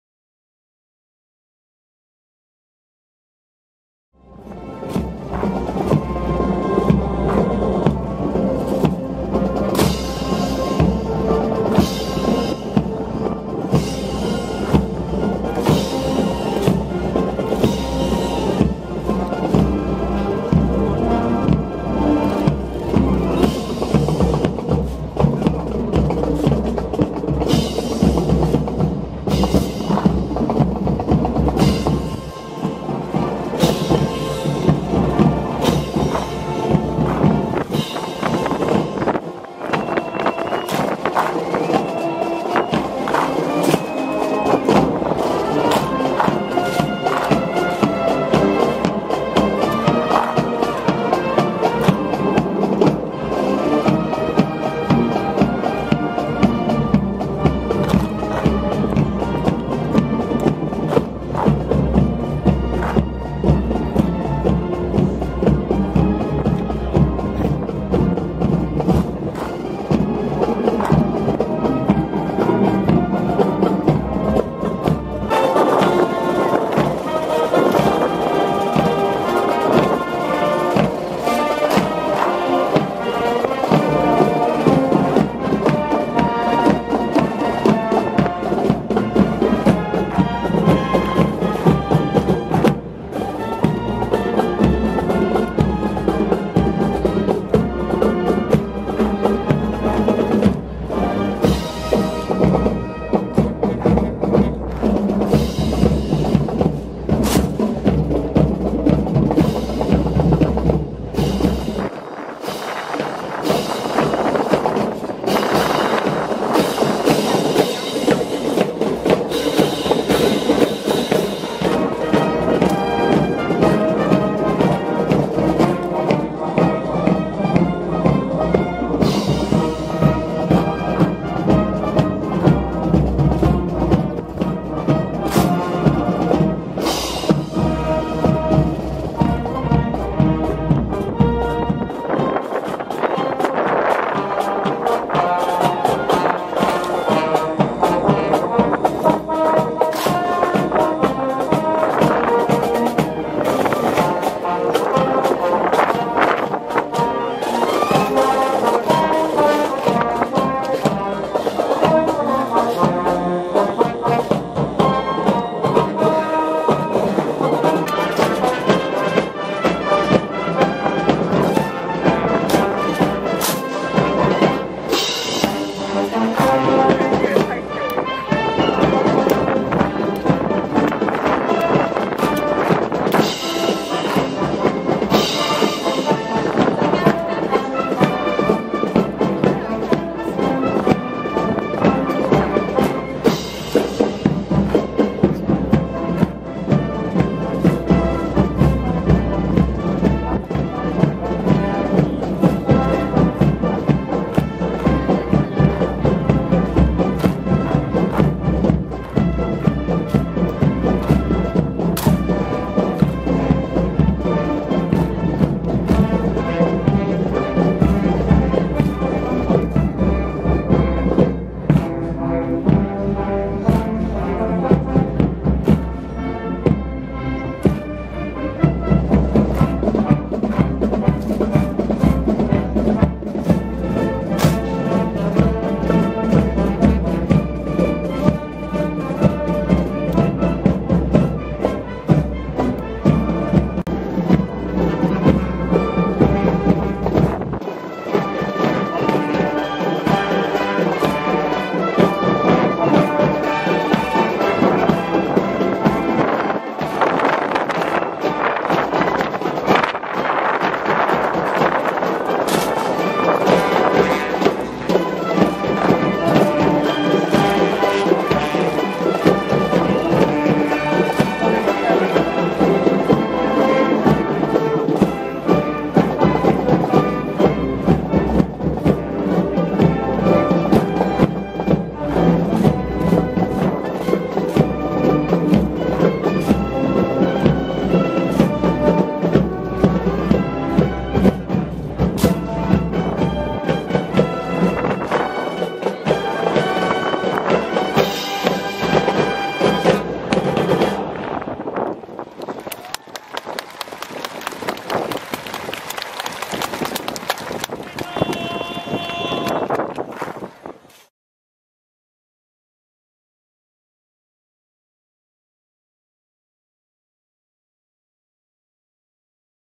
Taiwan - Republic of China Military Parade - Great Pagentry for National Day 2015
By serendipity I was at the Chiang Kai Shek Memorial on Sunday, October 4th, 2015.  A fellow came up to me and told... "there's going to be a performance!".  I'd seen the dozens of military buses parked round the Memorial.  Quite unusual.  So... it seems this was perhaps a dress rehearsal of the marching band and soldiers for National Day, October 10th.  Quite impressive.  These young lads with bayonets and tight-laced stomping boots.  A flashback in my mind of that rag-tag, perhaps "villainous", soldiers who arrived in Taiwan in 1949.